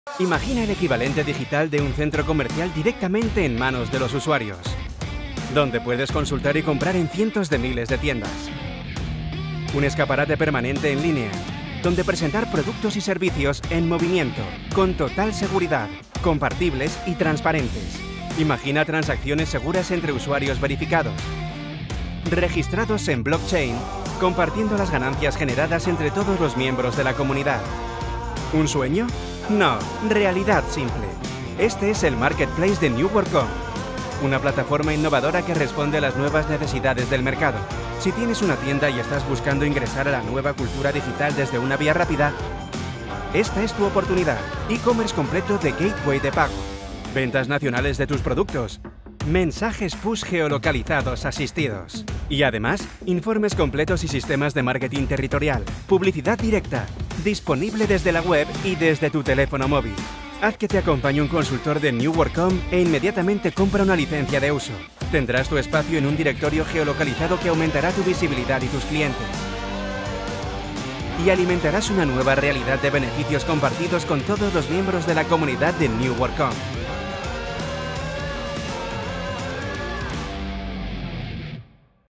Marketplace de Neworkom
Una plataforma innovadora, creada principalmente para su uso a través de dispositivos móviles, que se basa en compartir contenido y permitir intercambios comerciales, utilizando una nueva moneda virtual estable: y la tecnología Blockchain.

La plataforma se presenta como un gran centro comercial virtual en el que cualquiera puede comprar productos o servicios a precios competitivos en línea.
También es un sistema que permite mejorar el mercado local en particular, contribuyendo al apoyo de pequeños productores y comerciantes.

El Mercado de NEWORKOM está diseñado para ser un punto de referencia internacional y, por lo tanto, puede mejorar y garantizar el comercio entre empresas de todo el mundo.

Sigue nuestros canales